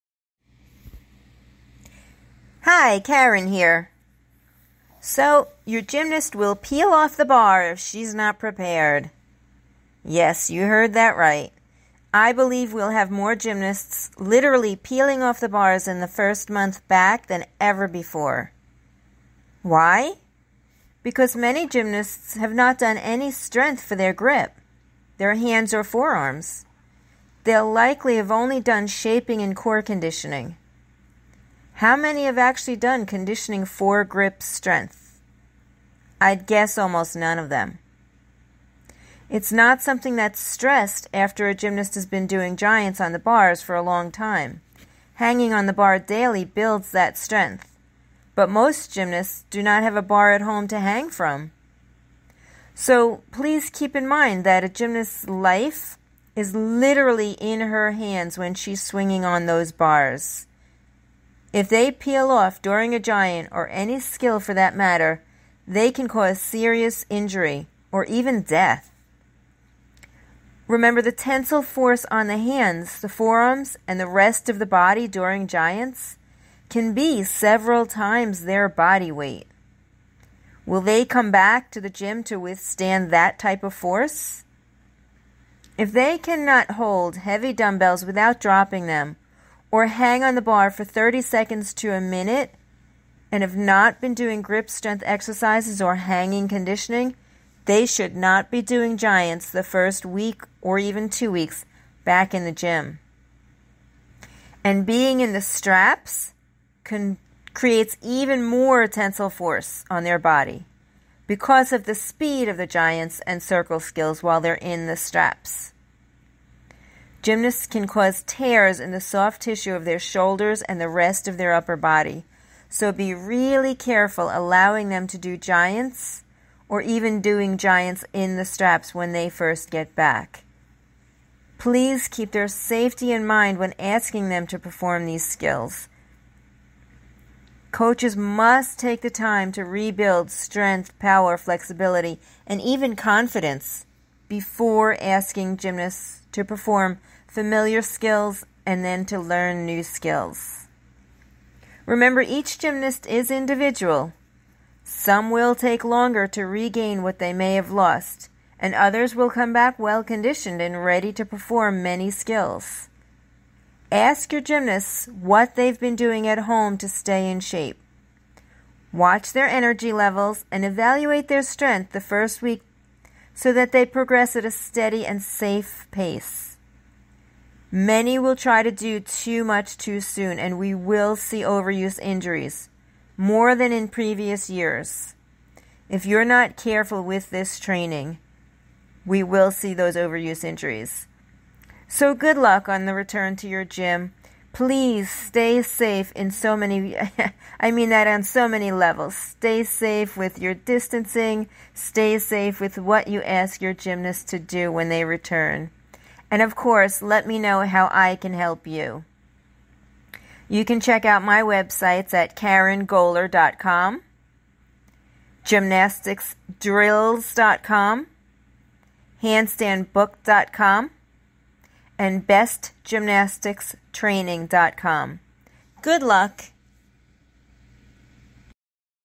Your Gymnast's Life is in Her Hands, Literally (made with Spreaker)
Your Gymnast’s Life is In Her Hands. Your Gymnast Will Peel Off the Bar if She is Not Prepared. I think we will have more gymnasts peeling off the bars in the first month back than ever before. Why? Because many gymnasts have not done any strength for their grip-hands and forearms. They will likely […]